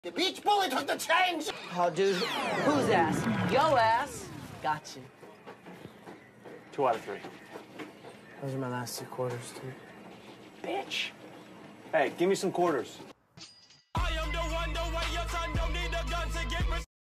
The beach bully took all the change! 💰️💰️💰️
Venice Heat is on the move! But, there is no change, the beach bully took all the change! 💰️💰️💰️ VeniceHeat @TJHoban